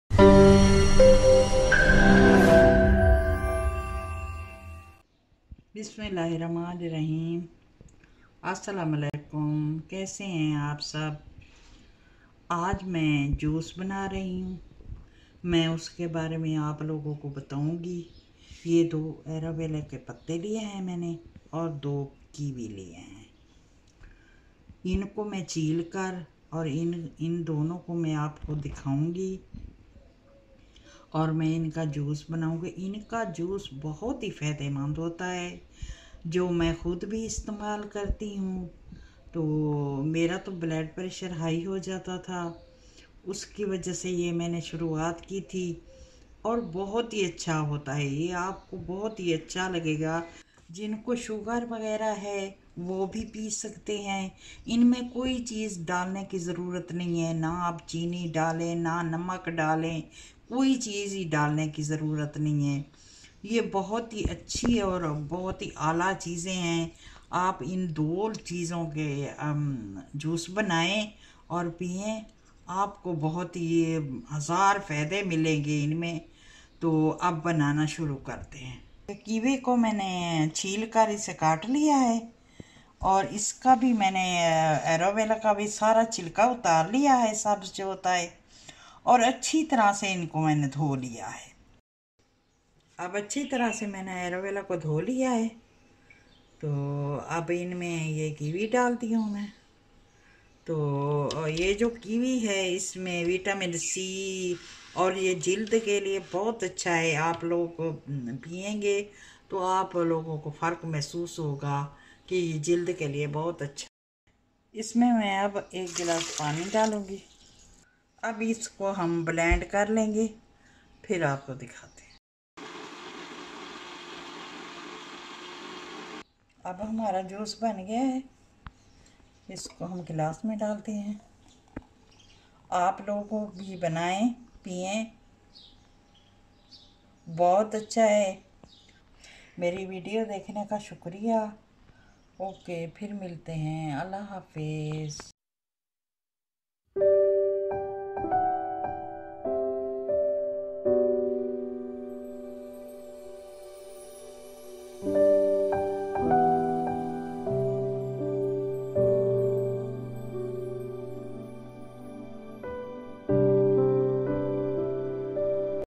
Aloe Vera Kiwi Juice Immunity Booster by Razia's Daily Life
Watch how to make Aloe Vera Kiwi Juice

Ingredients:
Aloe Vera
Kiwi

Enjoy eating :)

Thanks,
Razia's Daily Life.